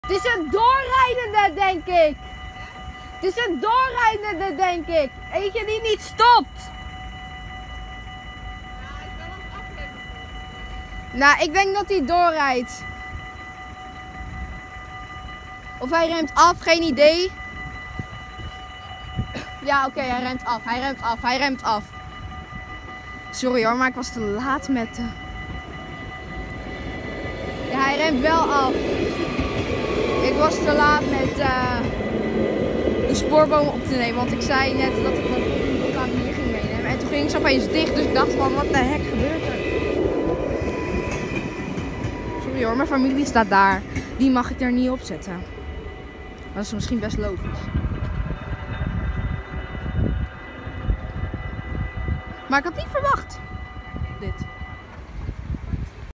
Virm komt aan in Heerhugowaard
Hoor mij schreeuwen aan het begin🤣🤣🤣🤣🤣🤣🤣😂😂😂😂😂😂. Ik stond nog in het midden daar dus ik had niet kunnen filmen dat de spoorbomen dicht gingen. Maar het draait erom dat ik de trein had kunnen filmen.

Video gemaakt op 3/5/2022